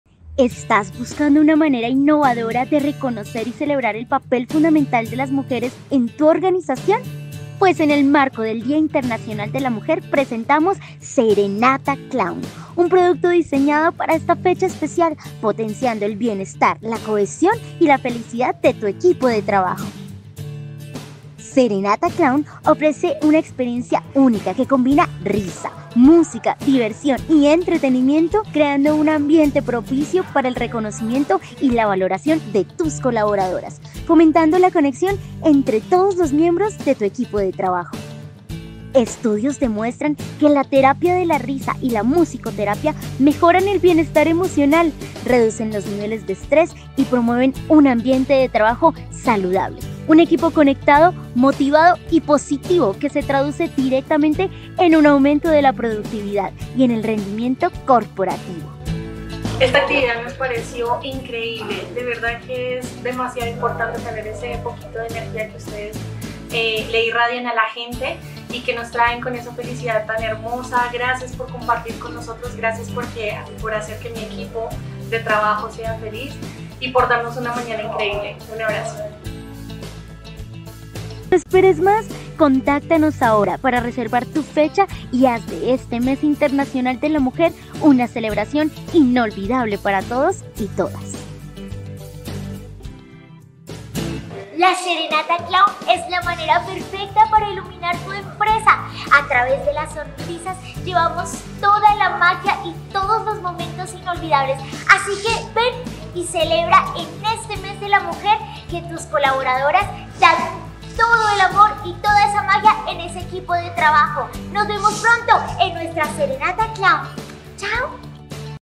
Serenata Clown | Fundación Doctora Clown
¿Está buscando una manera innovadora de reconocer y celebrar el papel fundamental de las mujeres en tu organización? En marco del día Internacional de la Mujer, presentamos "Serenata Clown", un producto especialmente diseñado para potenciar el bienestar, la cohesión y la felicidad de tu equipo de trabajo en esta fecha especial.  🏁📈

 Recibe información de nuestro Brochure Empresarial  ✨📈

¡Contáctanos ahora!